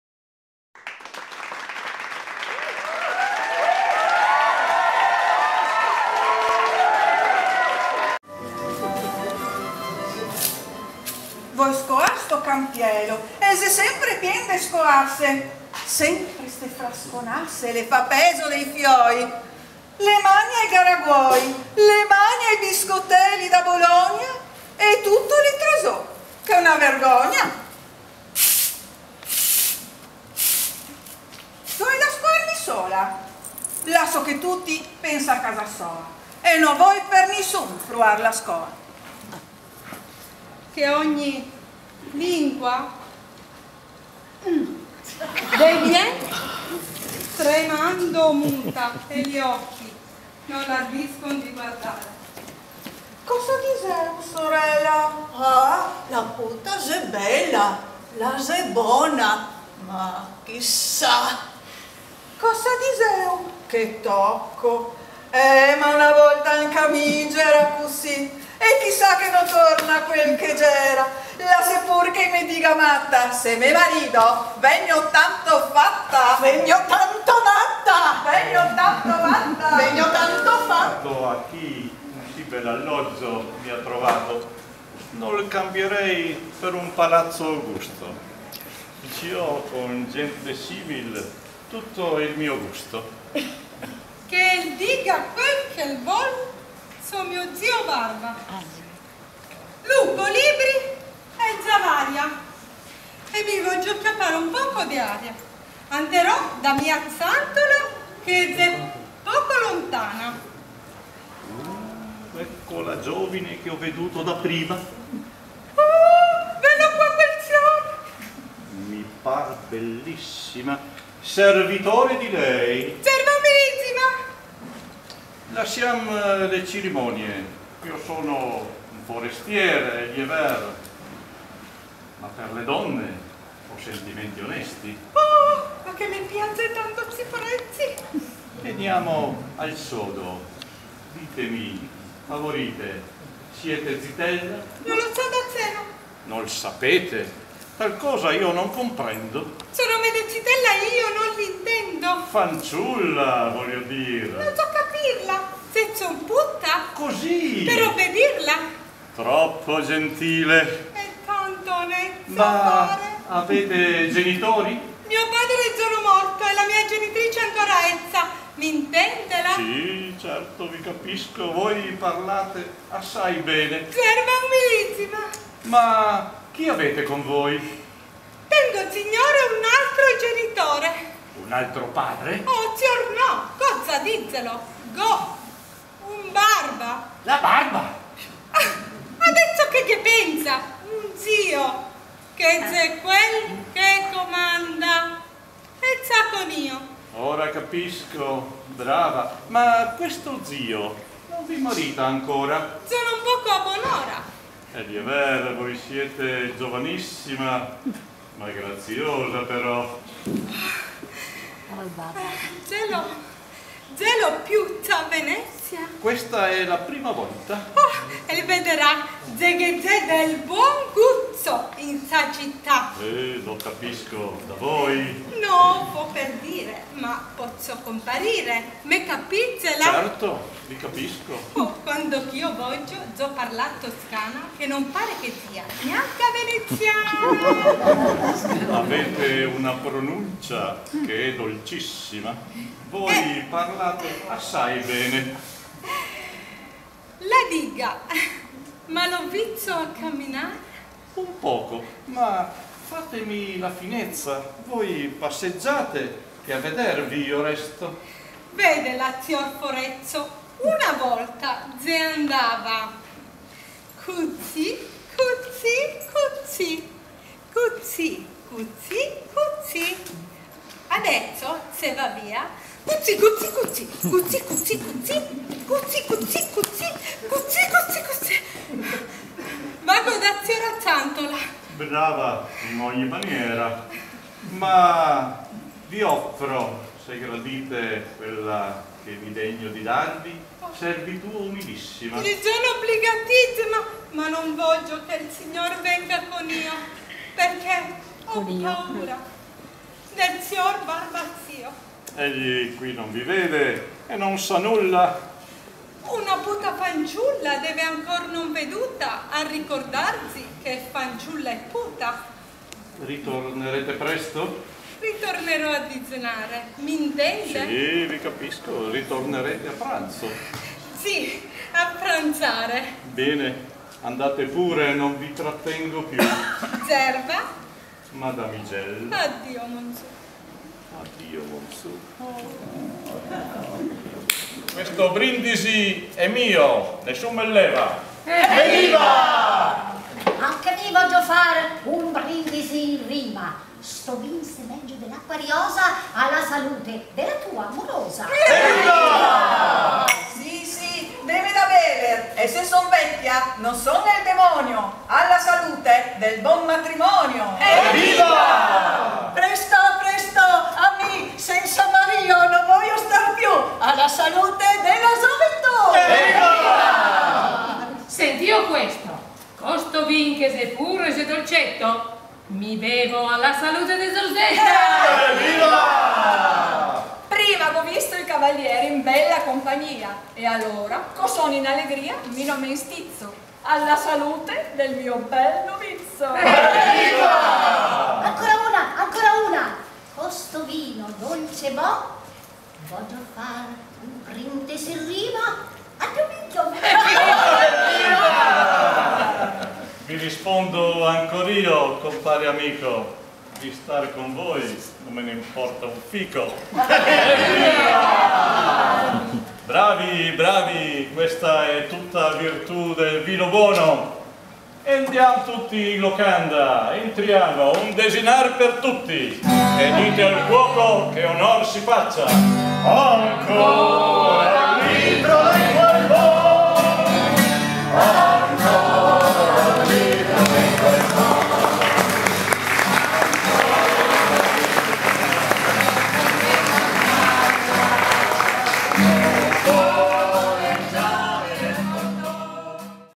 Grado - Teatro in Piazza - 2015 -"Tra canali e campielli" di C. Goldoni.
Grado - Teatro in Piazza - 2015.
La Compagnia UTE "senza arte, ne parte" di Cervignano del Friuli.
"Tra canali e campielli" di Carlo Goldoni. -
Regia di Tullio Svettini. (SPOT n. 4).